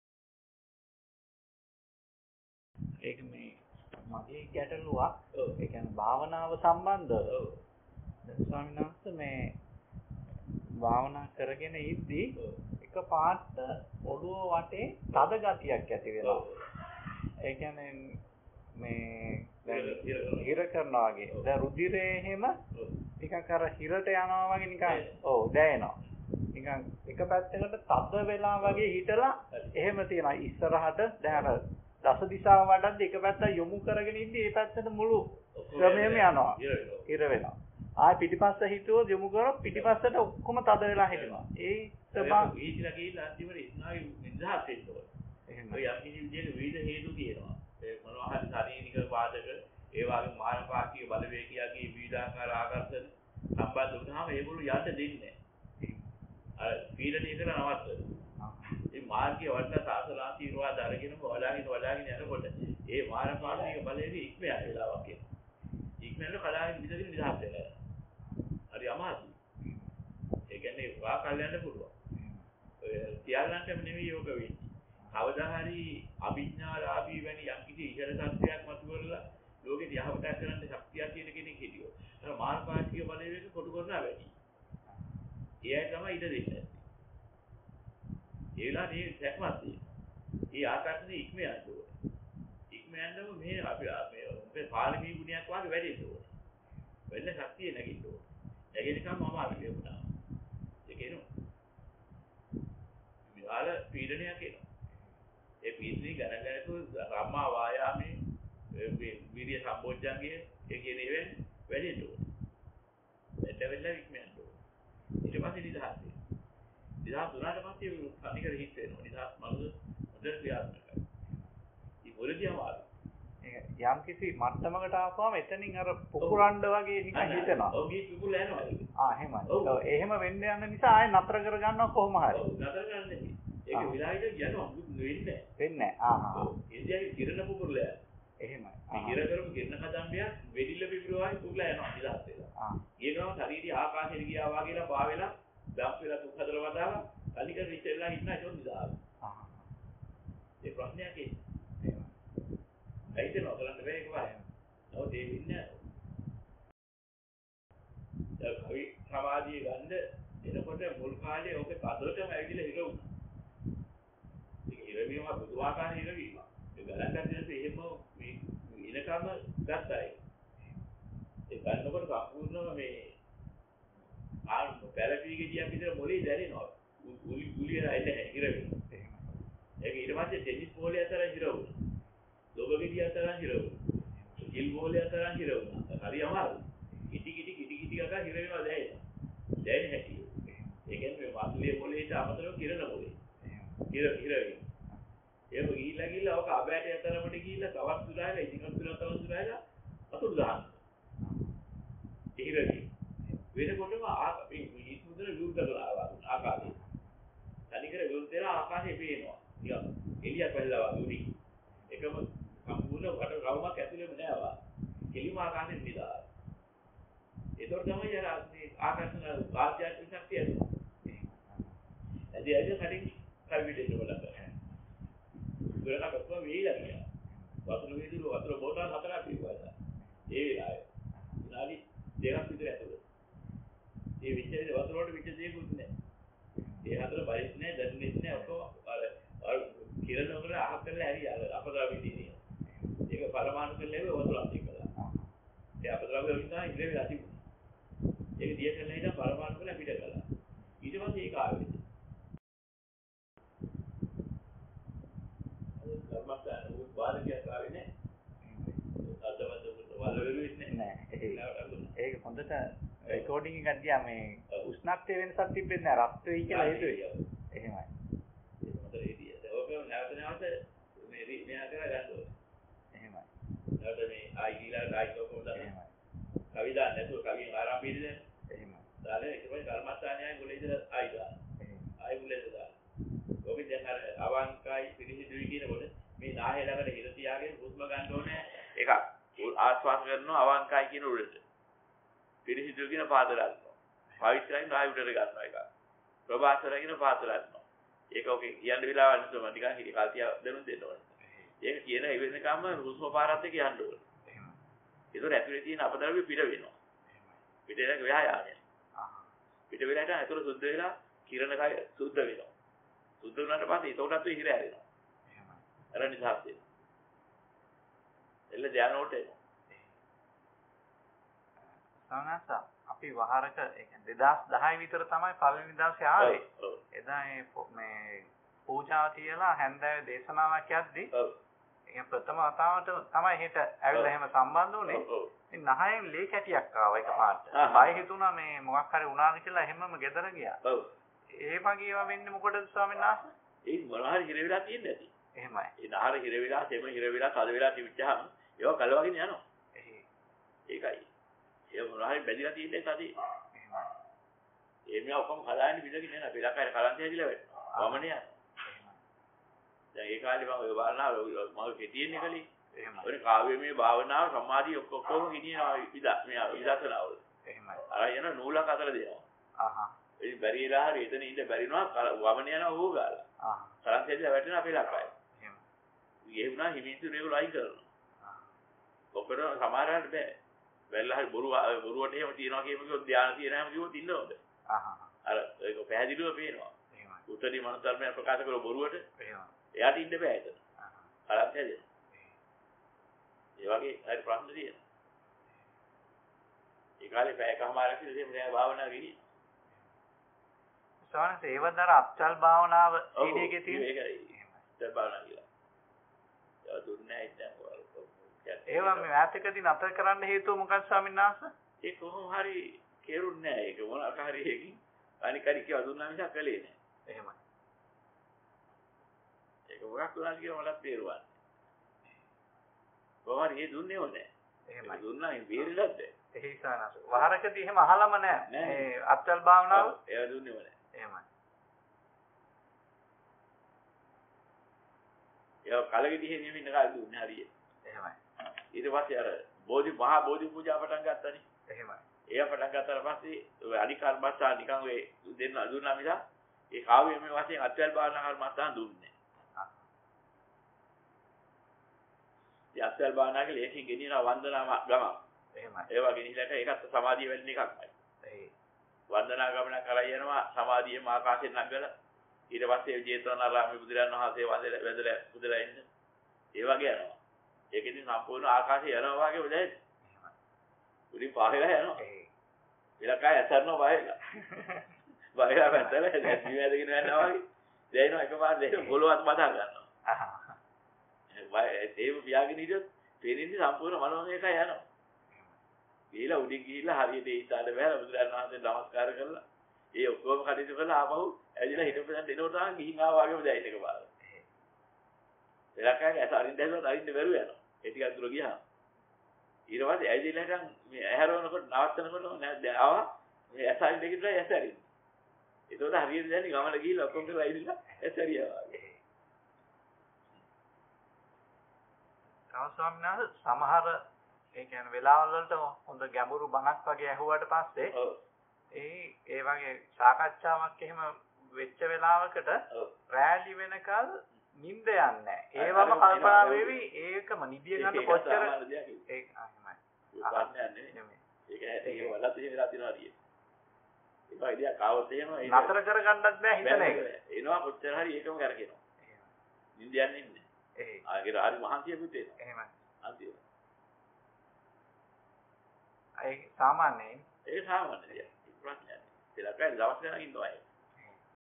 භාවනාවේදී ඇතිවන අපහසුතා ජය ගැනීම - Ven Waharaka Abayarathanalankara Himi – Bana
සදහම් සාකච්ඡා 3
දේශනා අංක 17